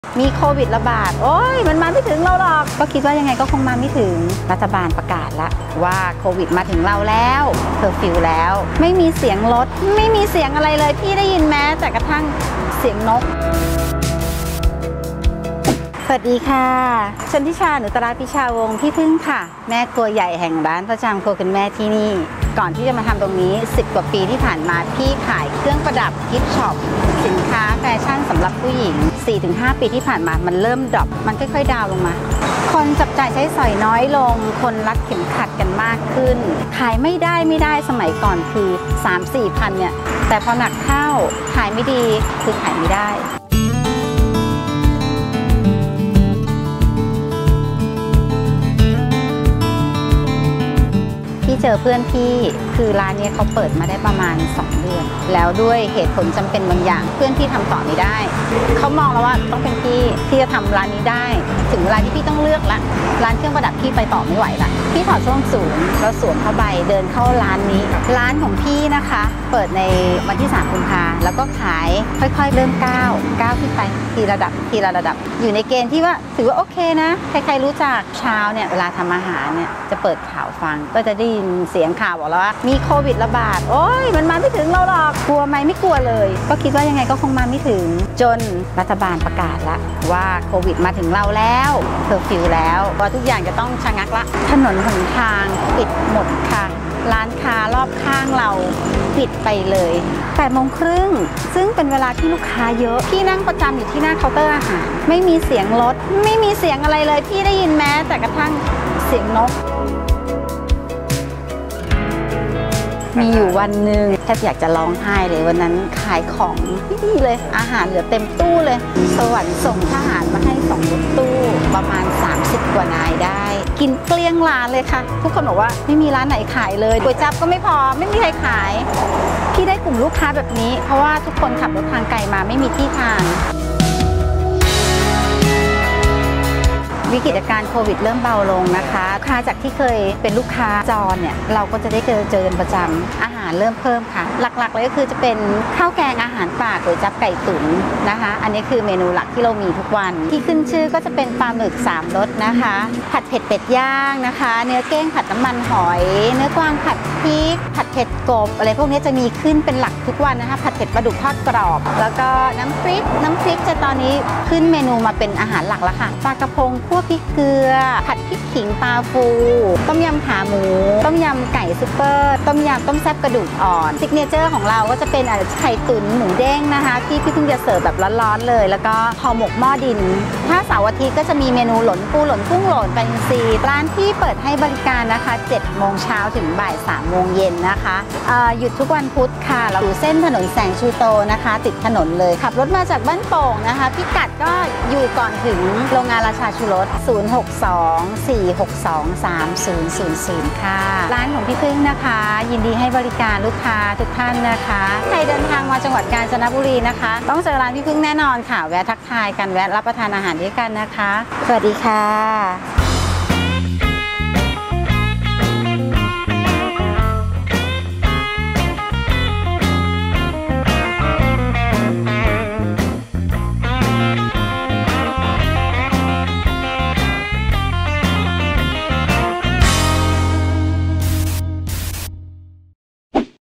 ร้านประจัมครัวคุณแม่ ข้าวแกงอาหารป่า
"จากแม่ค้าขายของสวยงามผันตัวมาเปิดร้านอาหารข้าวแกงดิ้นรนต่อสู้กับสถานการณ์ต่างๆมาได้ ปัจจุบันเมนูที่ร้านมีหลาหลายเยอะมากให้เลือกทานกันเชิญรับชมได้ในคลิปครับ"

ร้านเปิด 07:00 น. - 15:00 น. หยุดทุกวันพุธ

ร้านตั้งอยู่ เส้นถนนแสงชูโตมาจากบ้านโป่งไปทางกาญจนบุรีอยู่ก่อนถึงโรงงานราชาชูรสก่อนเข้าอำเภอท่ามะกา ร้านอยู่ซ้ายมือ อำเภอบ้านโป่ง จังหวัดราชบุรี